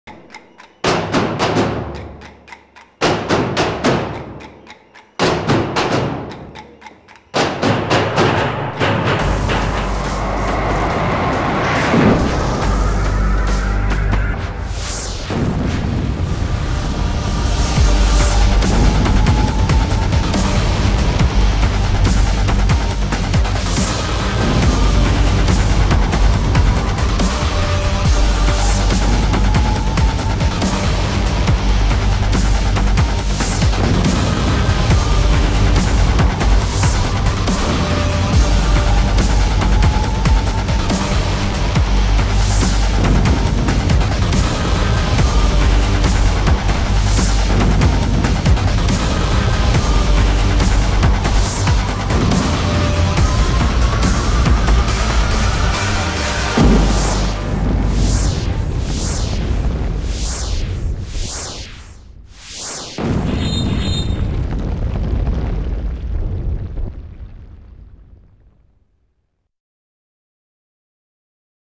TEASER EXTRACTION 2018
Présentation du Challenge National secours-routier des sapeurs-pompiers au Vélodrome de St Quentin-en-Yvelines du 24 au 26 mai 2018.3